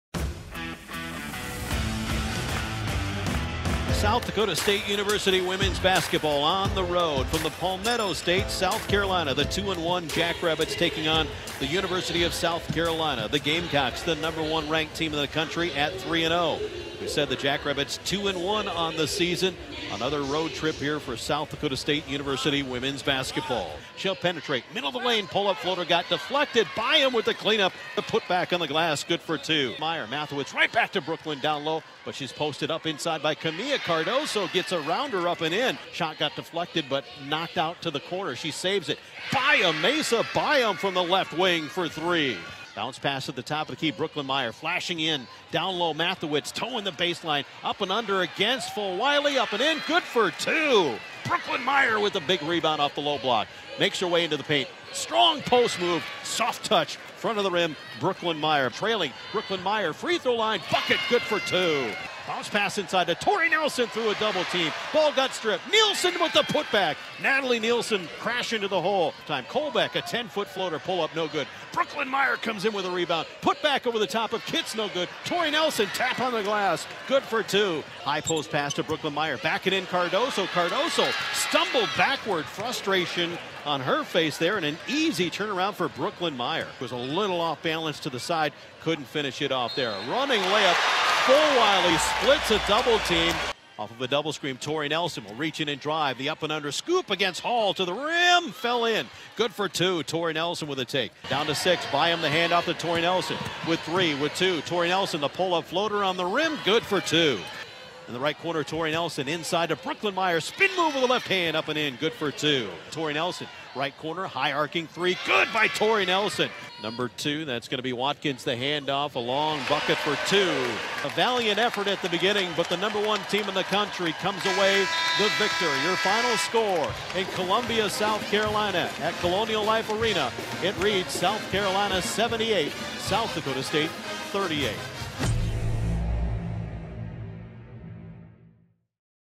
Women's Basketball Highlights at South Carolina (11.20.2023)
The Jacks fought hard, but fell to No. 1 South Carolina 38-78.